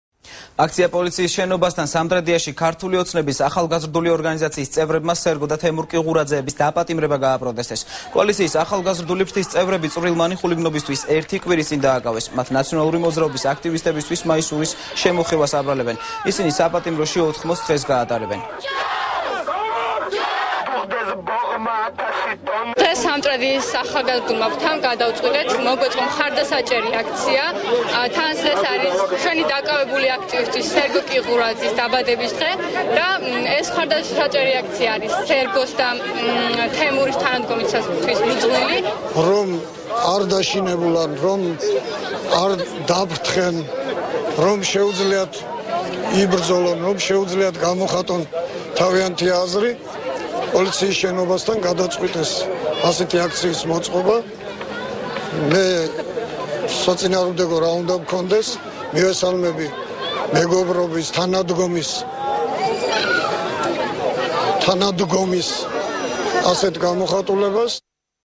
ახალი 3 | აქცია სამტრედიაში | 26.08.12
აქცია პოლიციის შენობასთან-- სამტრედიაში „ქართული ოცნების„ ახლგარდული ორგანიზაციის წევრებმა სერგო და თემურ კიღურაძეების დაპატიმრება  გააპროტესტეს. კოალიციის ახალგაზრდული ფრთის წევრები  წვრილმანი ხულიგნობისთვის ერთი კვირის წინ დააკავეს. მათ ნაციონალური მოძრაობის აქტივისტებისთივს მაისურის შემოხევას აბრალებენ. ისინი საპატიმროში ოთხმოც დღეს გაატრებენ.